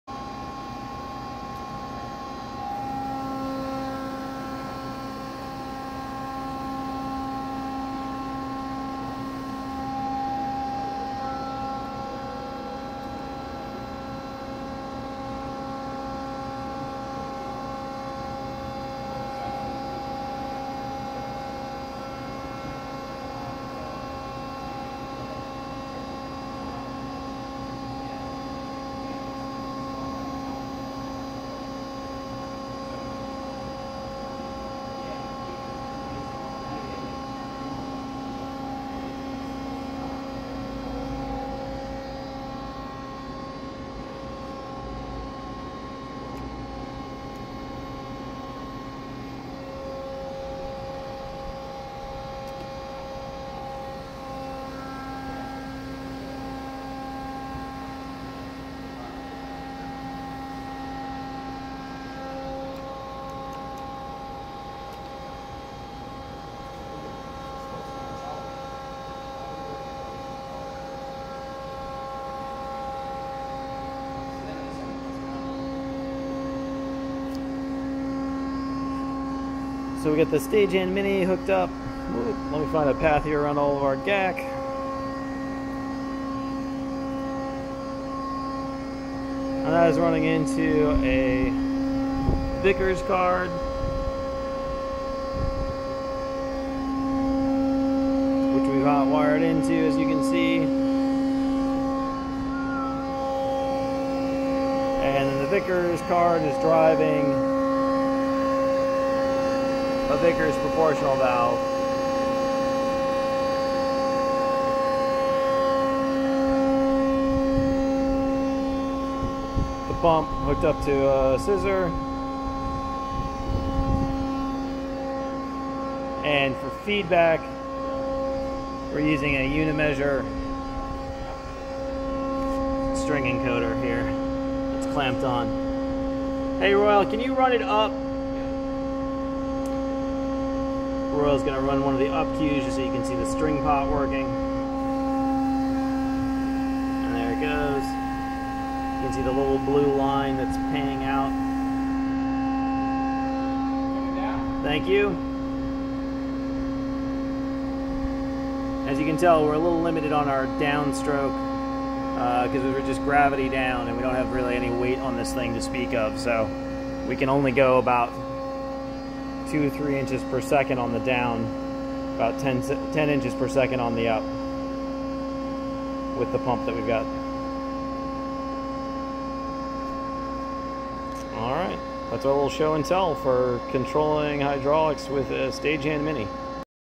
Single-acting hydraulic lift with Spikemark
Here's quick video showing a test of a Stagehand Mini controlling a single-acting lift with a Vickers proportional valve.